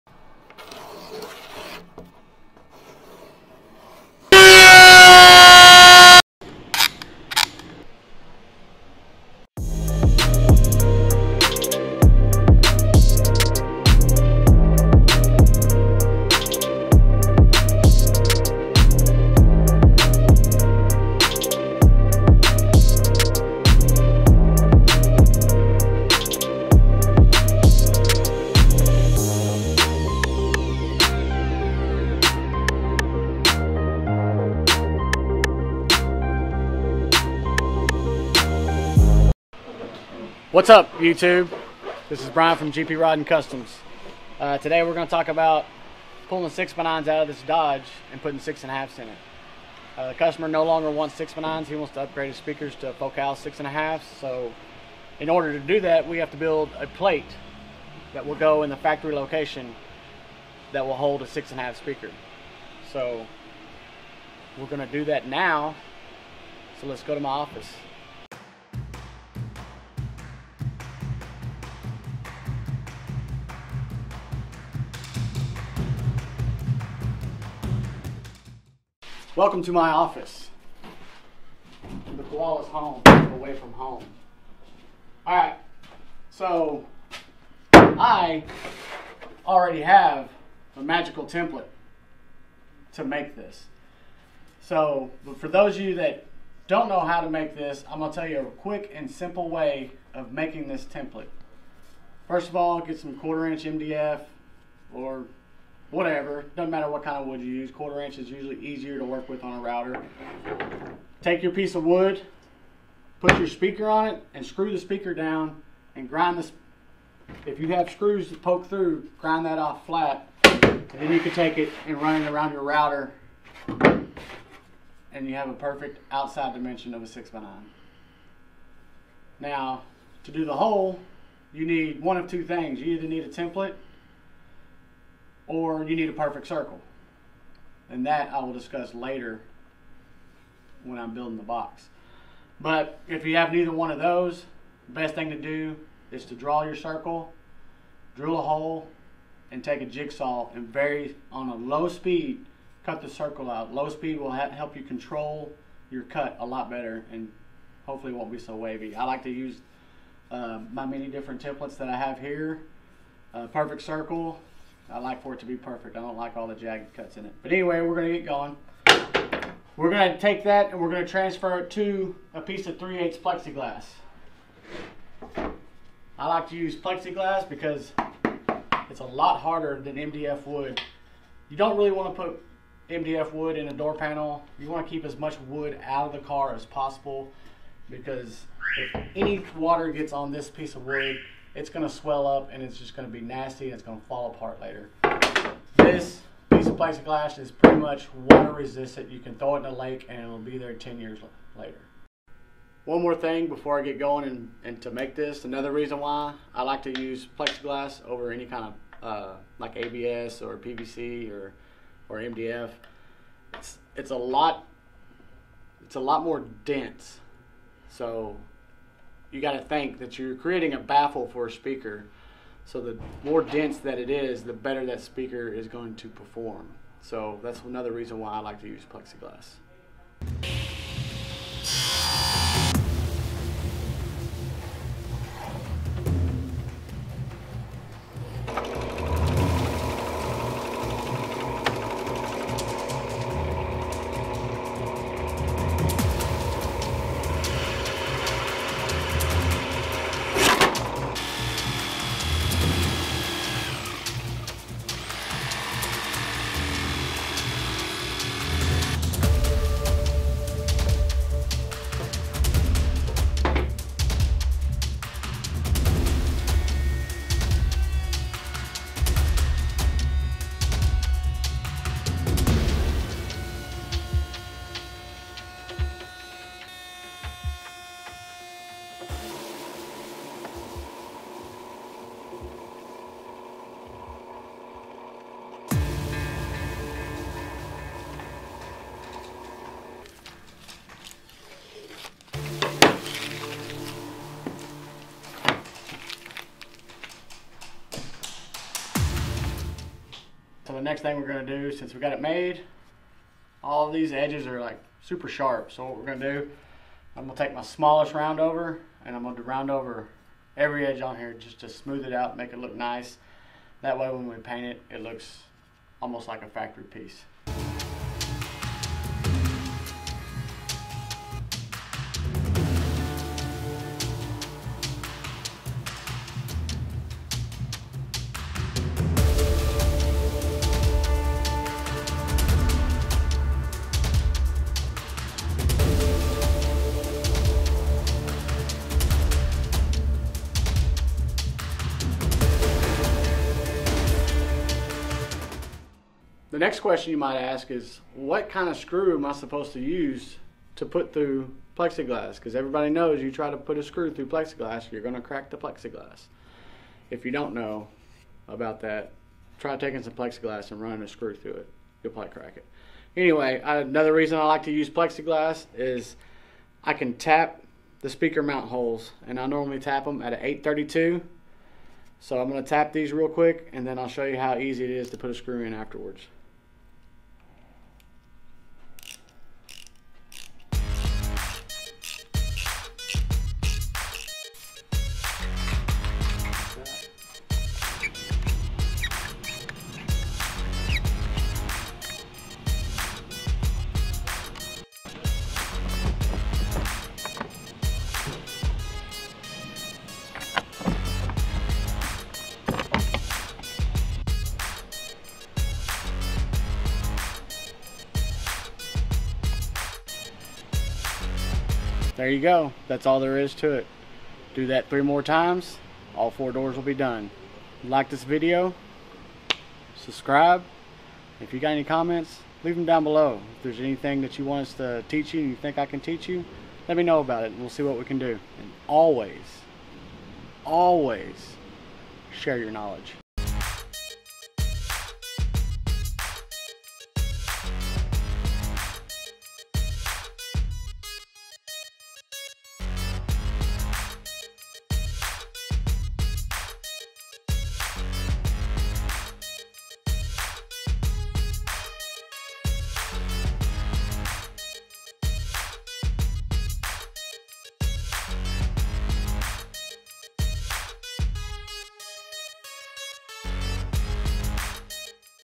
Custom Speaker Adapter Plate 6x9 | Teach It Tuesday
Bryan Hale @The_KoalaMaster74, walks you through the process of build an adapter plate for a 6.5" speaker going into a 6x9 factory location.

These plates are going to hold 6.5" Focal midrange drivers in a 2019 Ram Truck.

If you have any question for Bryan or comments leave them below and we will make sure it gets you an answer.